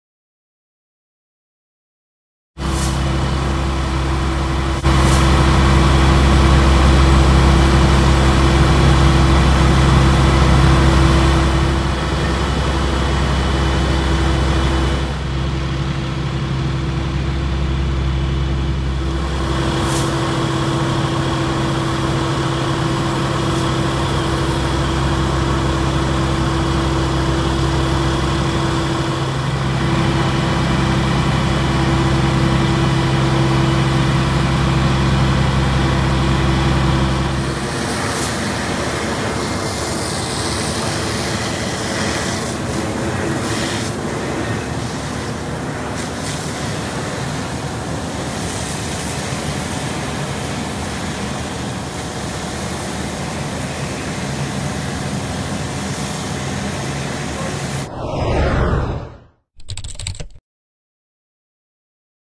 Fire at Agawam lumber yard
Fire destroyed a barn at Lucia lumber in Agawam